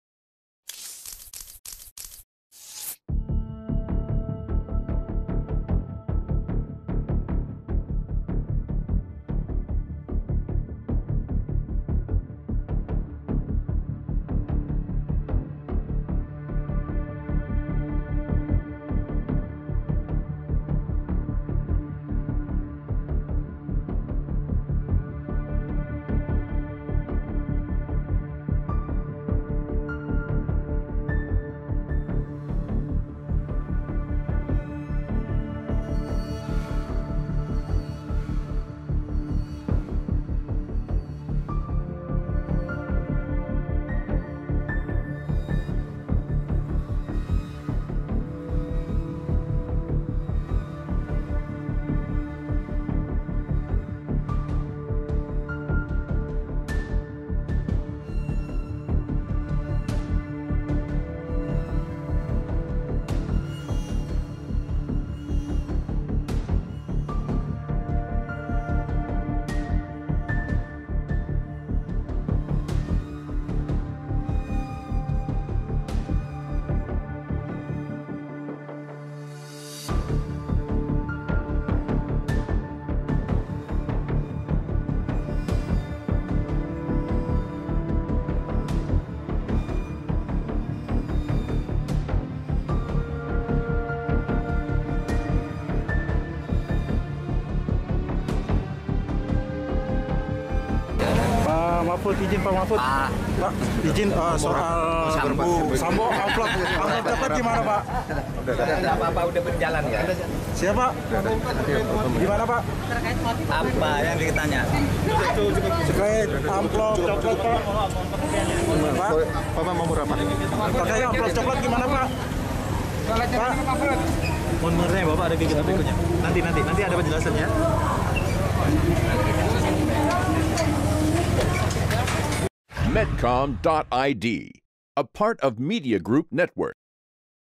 Sempat Beri Amplop Coklat, Sambo Coba Suap LPSK
Lembaga Perlindungan Saksi dan Korban (LPSK) baru-baru ini memberi informasi mengejutkan dengan menyebut dua petugas LPSK sempat disodorkan dua amplop berwarna coklat oleh seseorang yang diduga orang suruhan Irjen Ferdy Sambo saat lpsk mengunjungi kantor Divpropam Polri. Kedua amplop coklat dengan tebal sekitar 1 cm itu diduga berisi uang.

Wakil Ketua LPSK Edwin Partogi Pasaribu lah yang menjelaskan kejadian yang dialami oleh dua orang petugas LPSK saat mendatangi kantor Irjen Ferdy Sambo 13 Juli 2022 lalu. Edwin menyebut kala itu usai kejadian penembakan di rumah dinas Irjen Sambo, petugas LPSK mendatangi kantor Irjen Sambo di Mabes Polri, Jakarta. Kehadiran LPSK tersebut untuk berkoordinasi terkait tewasnya Brigadir J.

Dalam kunjungan itu, dua petugas LPSK yang datang disodorkan dua amplop dengan tebal sekitar satu sentimeter yang diduga berisi uang. LPSK tidak mengetahui apa maksud pemberian amplop tebal tersebut, namun pria berseragam hitam yang menyodorkan amplop tersebut diduga merupakan orang suruhan Irjen Ferdy Sambo. Pihak LPSK menegaskan kedua petugas  itu tidak membuka dua amplop berwarna coklat itu dan langsung mengembalikannya.

Sementara itu Menteri Polhukam Mahfud MD tidak memberi berkomentar banyak saat ditanya awak media terkait dugaan suap yang diduga dilakukan Ferdy Sambo kepada LPSK. Mahfud hanya menegaskan kasus pembunuhan yang melibatkan Irjen Ferdy Sambo saat ini sedang berjalan.